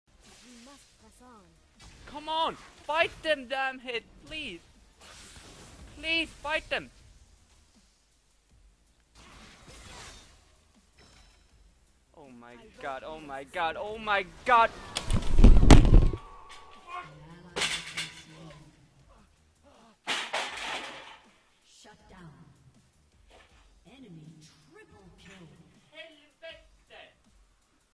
RAGE QUIT (Throw chair)
Ok, here is the story.
Ranked games we are 5 they are 4. Should be an  easy win right?
WRONG!
My team feed and feed, refuse to group, teamfight, push.
Just keep solo and die.
I'm doing ok. 7-2 as ADC.
I was annoyed the whole game trying to get my team to work together and this was just too much for me.
(I know I got caught etc but still)

Take care bros!